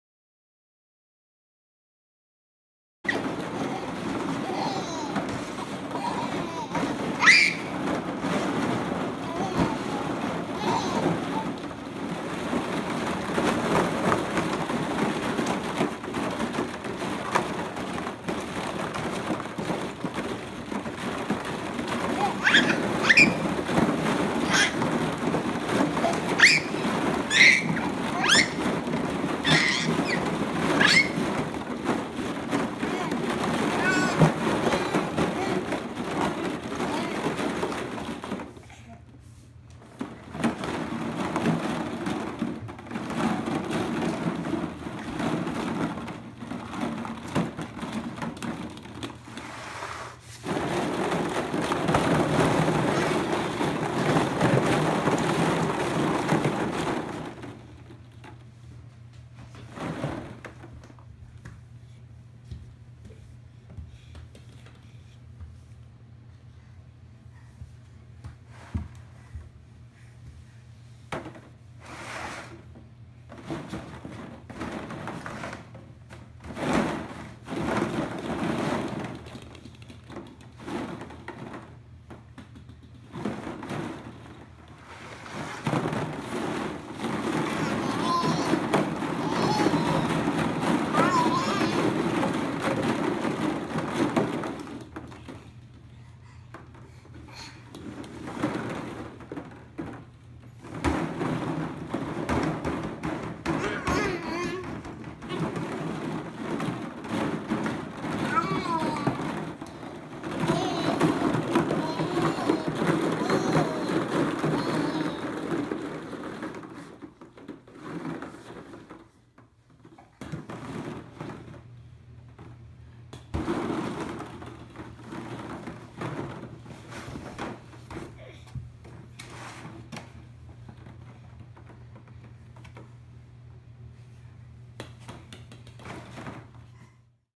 Swimming Pool Ball Pit
Our 1 year old and his first swimming pool ball pit. Anyone up for 250 ball pickup?